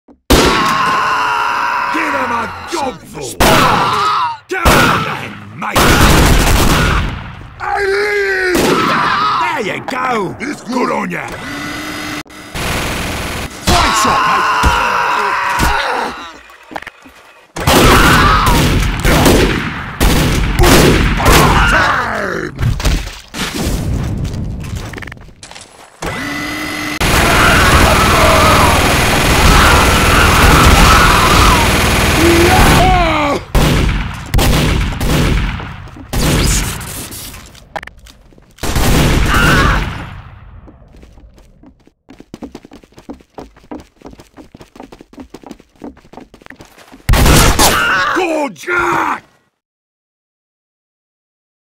[SFM] Testing out bots
This started out as a Simple screenshot test,then turned into this.
So much more laggy ingame!